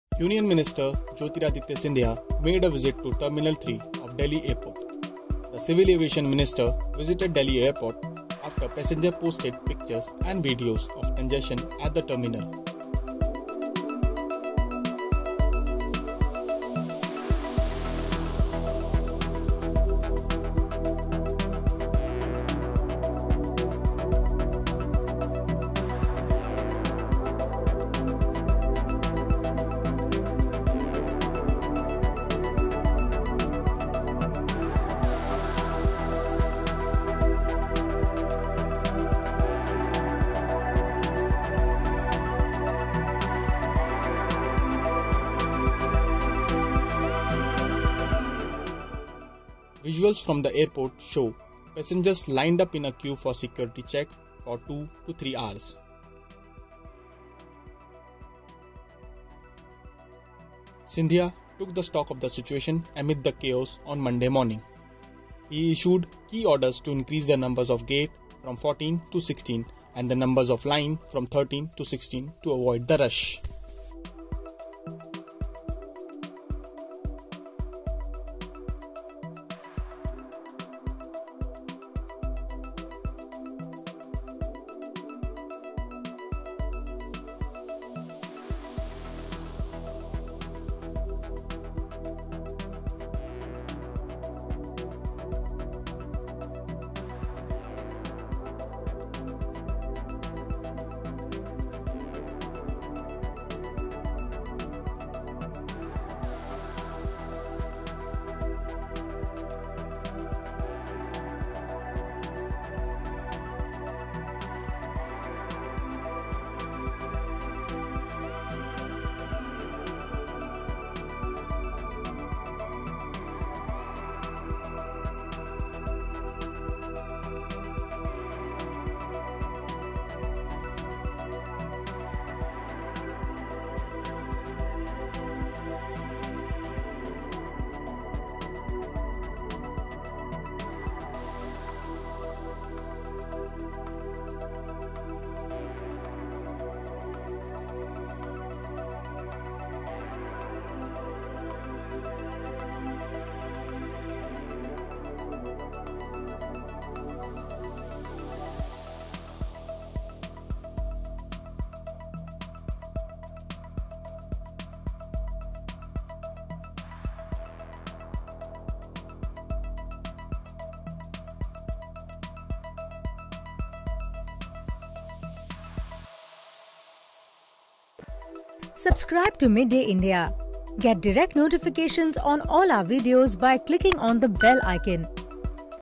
Jyotiraditya Scindia Visits Delhi Airport Amid Chaos, Issues Key Orders
Union Minister Jyotiraditya Scindia made a surprise visit to terminal 3 of Delhi Airport. The civil aviation minister visited Delhi Airport after passengers posted pictures and videos of congestion at the terminal. He issued key orders to increase the number of gates from fourteen to sixteen, and the number of lines from thirteen to sixteen to avoid the rush.